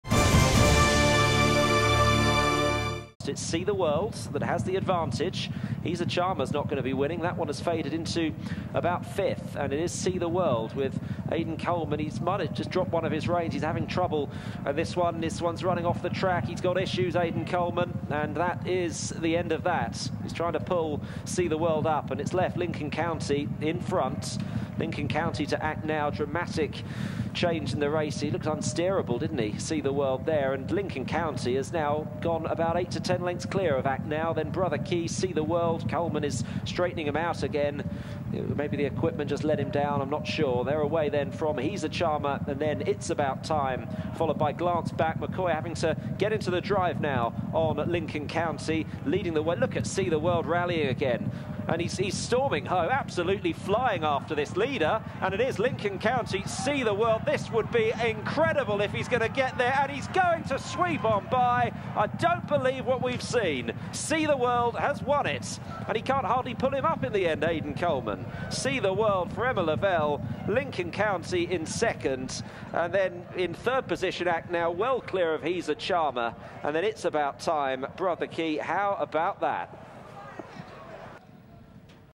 Have you seen anything like it? Amazing horse race at Wincanton
Aidan Coleman and See The World win a Wincanton bumper from a near-hopeless position in a quite amazing horse race and a truly remarkable finish at Wincanton.

You have to see it to believe it!